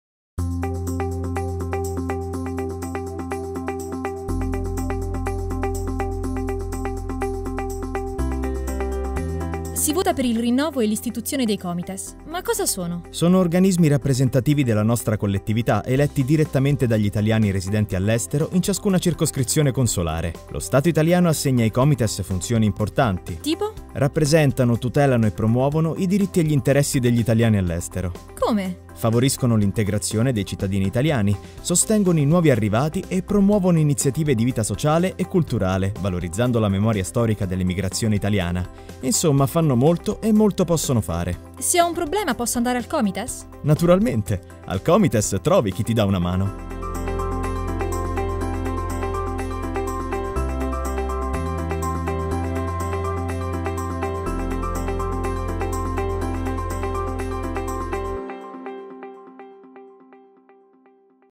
1 COMITES, COSA SONO
Se tenho um problema, posso me dirigir ao Comites?  - pergunta uma voz feminina. A Resposta é "sim", no primeiro de uma série de vídeos promocionais distribuídos pelo governo italiano, através da rede consular, com o objetivo de informar sobre as eleições em curso. Não há informação de que tipo de problema podem os ítalo-descendentes levar aos Comites, mas a sugestão é que "os Comites fazem muito e muito eles podem fazer".

Se tens algum problema, vá aos Comites e eles te darão uma mão, repete a mesma voz antes de aparecer a logomarca do "Ministero degli Affari Esteri e della Cooperazione Internazionale". O material foi produzido pela Agência 9Colonne, uma das muitas na Itália que recebem subsídios oficiais.